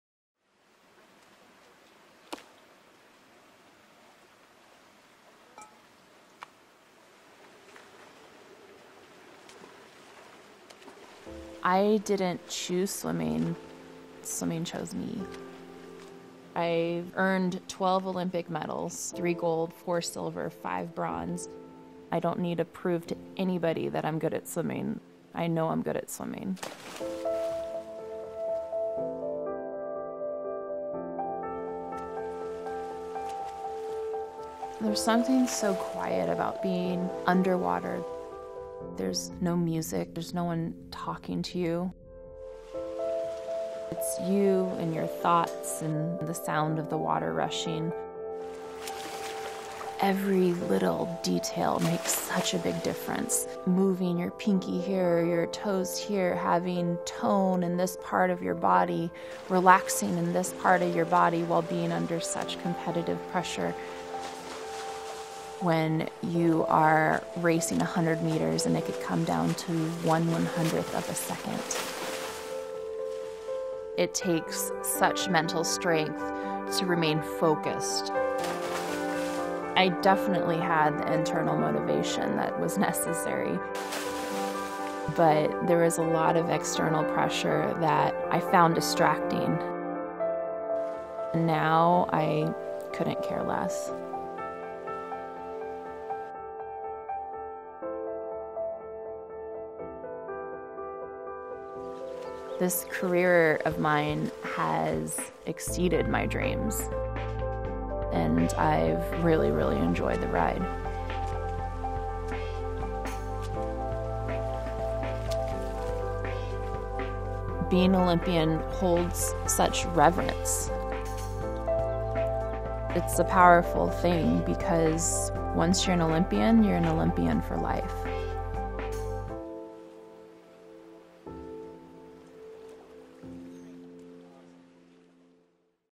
Olympic Swimmer Natalie Coughlin Talks Training Mentally and Physically | SELF
Olympic swimmer Natalie Coughlin opens up about her motivations and distractions as she trains for the Olympic games. As a 12-time medalist, she explains how the smallest details can make milliseconds of difference while competing and how her own confidence and the calm of the water combats external pressures.

ABOUT SELF
Daily health, fitness, beauty, style advice, and videos for women who want to achieve their personal best in life.